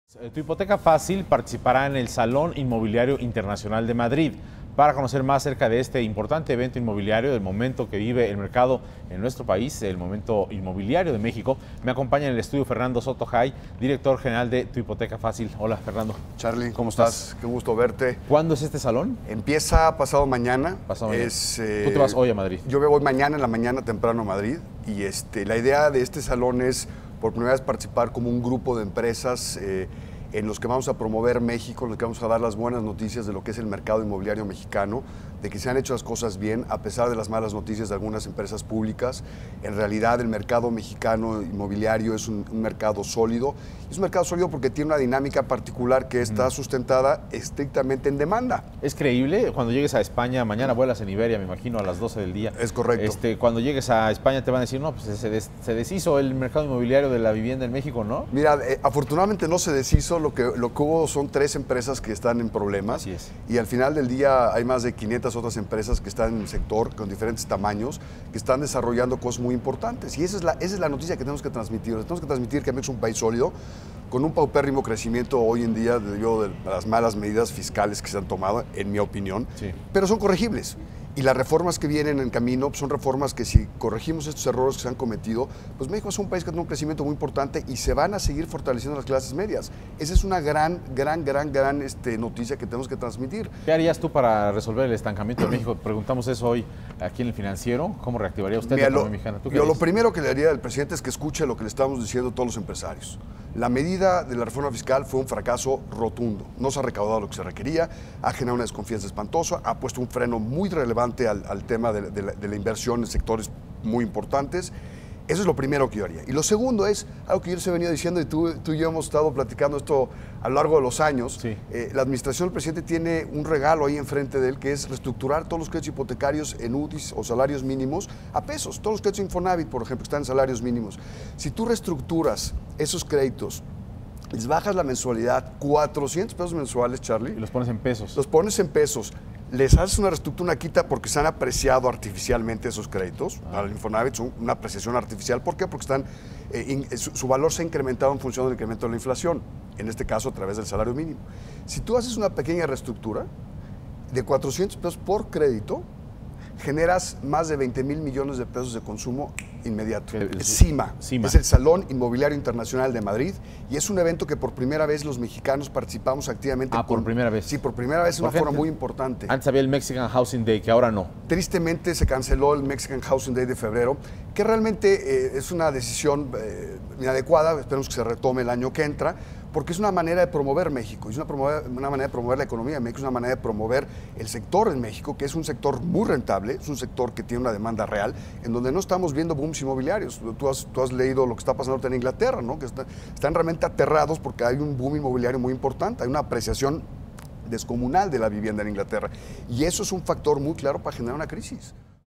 Tu Hipoteca Fácil, empresa mexicana participa en Madrid
Tu hipoteca Fácil participará en el salón inmobiliario internacional de Madrid, el director de dicha empresa dará a conocer el momento del mercado inmobiliario en México y su participación en España.